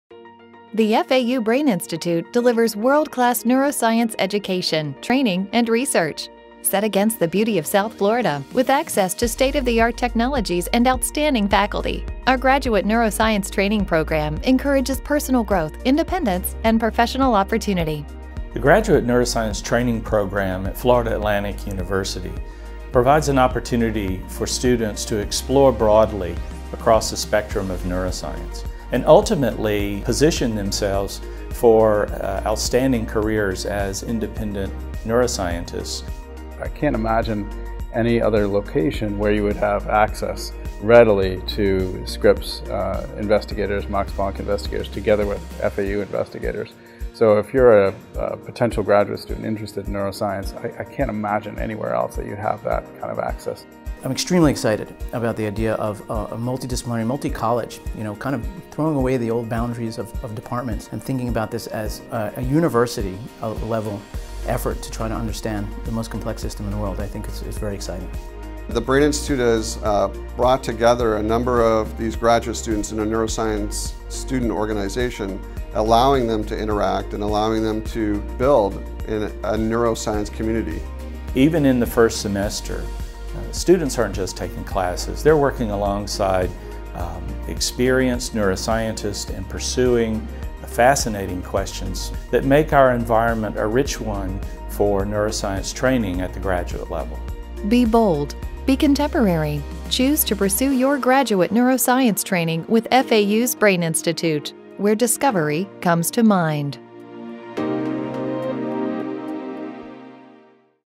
FAU Graduate Neuroscience Training Program (GNTP)
FAU Brain Institute’s Graduate Neuroscience Training Program (GNTP) unites scientists, educators and students from three FAU Ph.D. programs (Integrative Biology-Neuroscience, Complex Systems and Brain Sciences, Experimental Psychology), and from two internationally-recognized research institutes (the Max Planck Florida Institute for Neuroscience and the Scripps Research Institute), into an outstanding, one of a kind training opportunity for those at the beginning stages of their neuroscience careers.